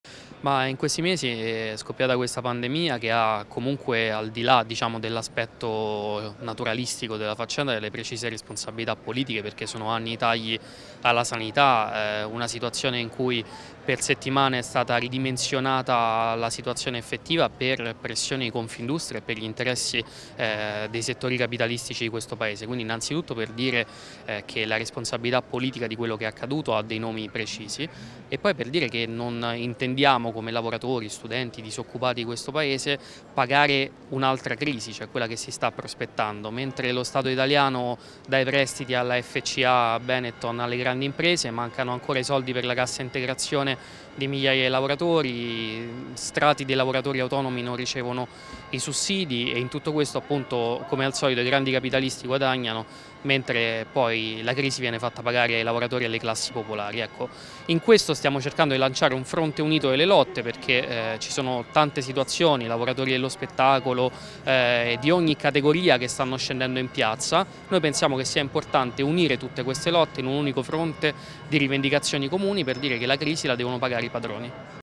Mustillo del Partito Comunista: “La crisi la devono pagare i padroni”
(Agenzia Vista) - Roma, 7 Giugno 2020 - “La crisi la devono pagare i padroni”, queste le parole di Alessandro Mustillo, Partito Comunista, al termine della manifestazione promossa dal Fronte della Gioventù Comunista in piazza San Silvestro a Roma.